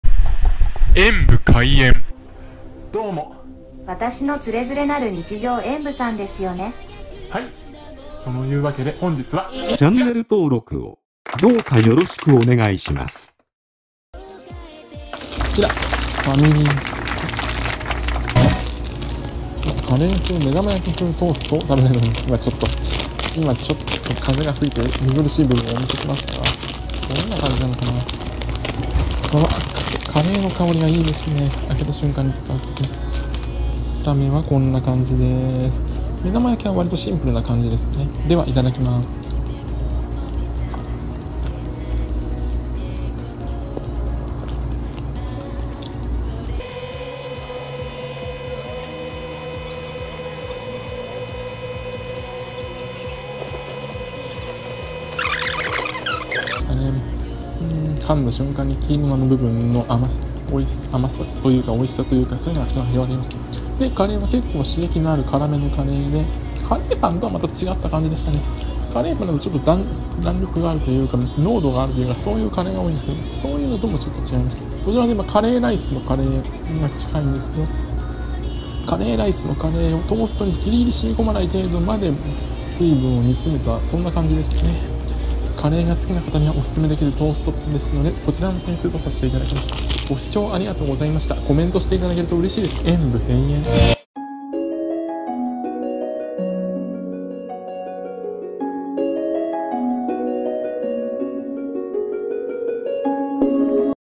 ファミマのカレーパンとは又違うカレートーストを食べてみた。ファミマ
ファミリーマートのカレートーストを食べてみました。カレーパンとは又違いますね～そして目玉焼き風……って、風とは何？